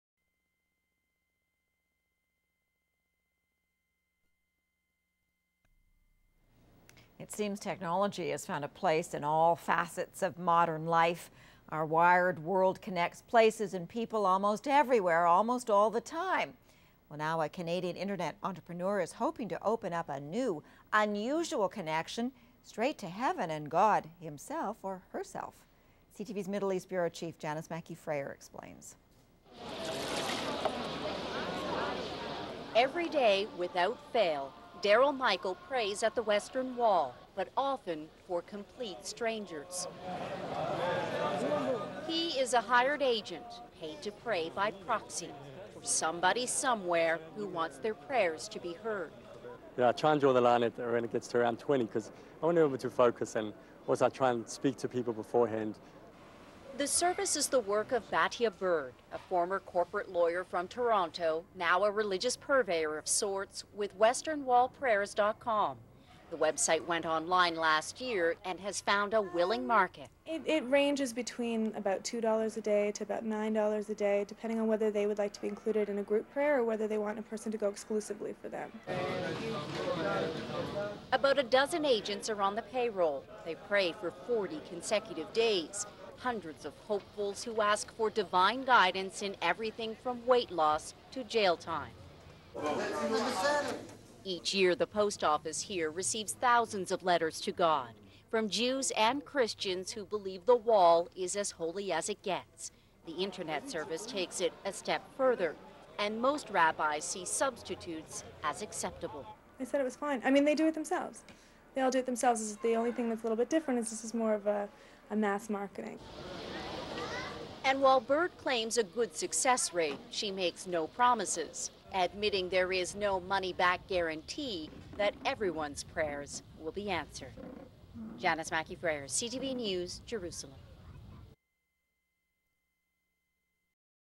Western Wall Prayers - Canadian Television (CTV) broadcast
40 days of prayer for you at the Western Wall (kotel)in the Old City of Jerusalem by a Torah observant Jew.